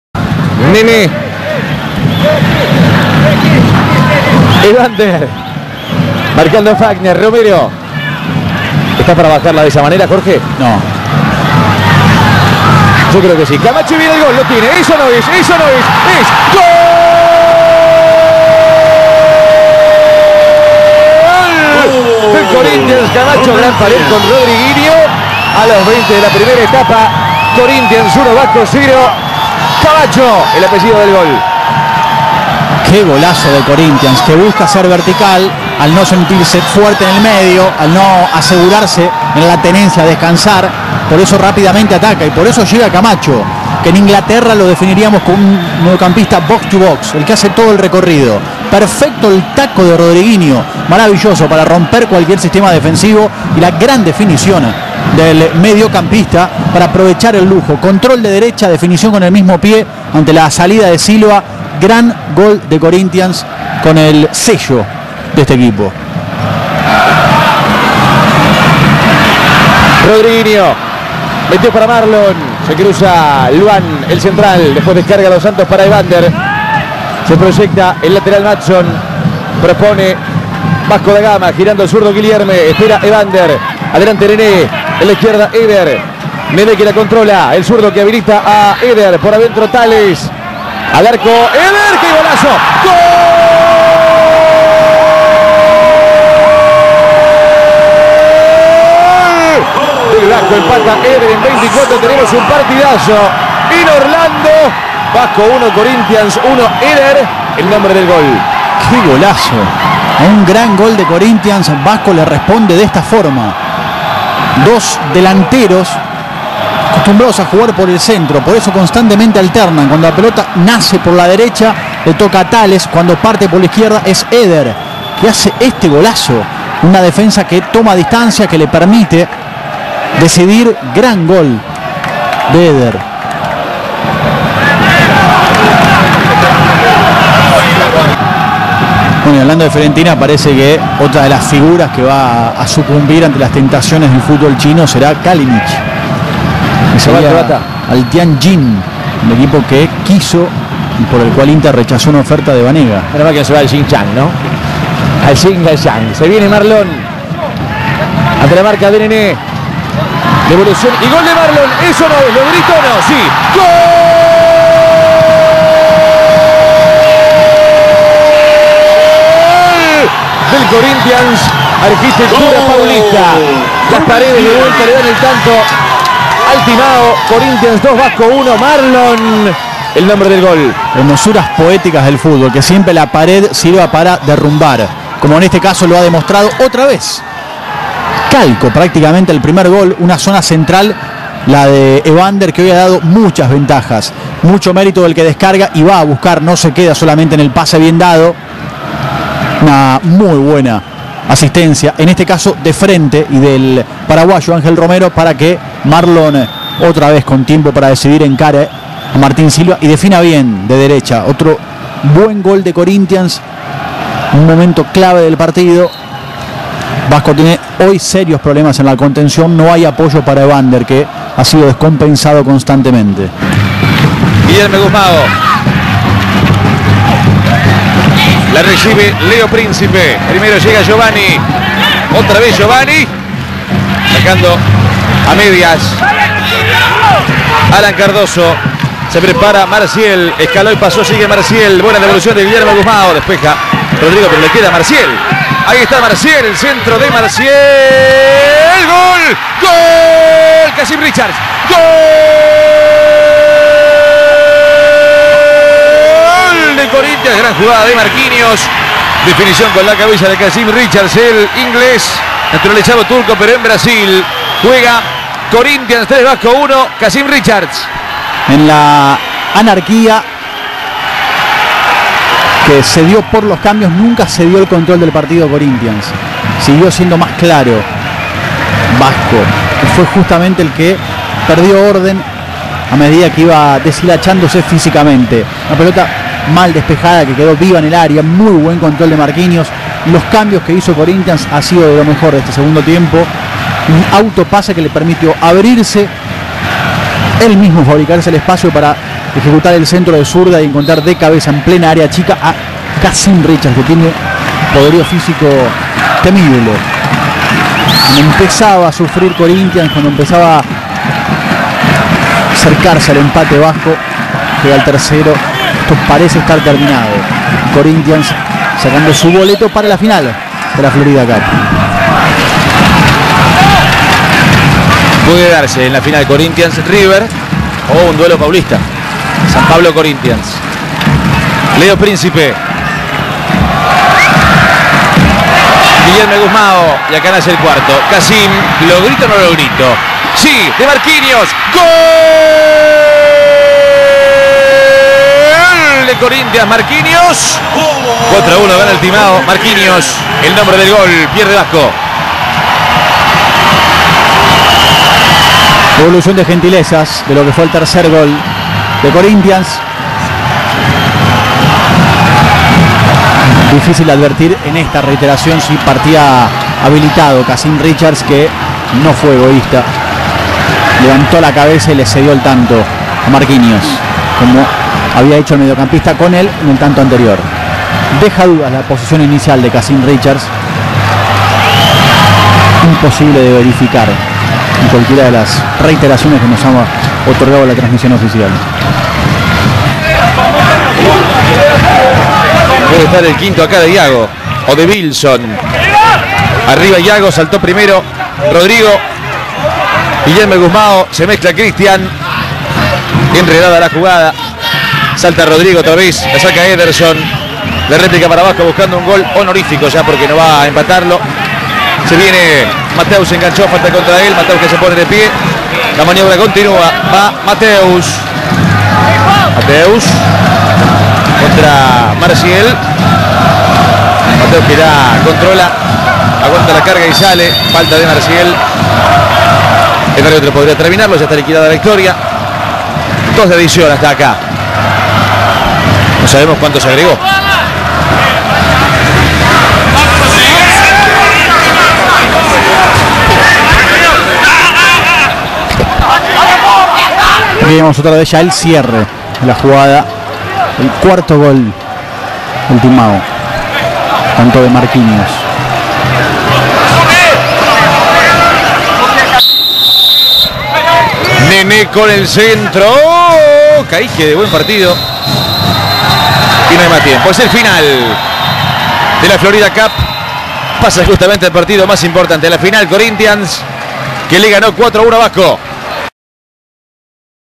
Vasco Da Gama 1 Corinthians 4 (Fox Sports) Florida Cup 2017
Los goles del partido con relato de Pablo Bari Fox Sports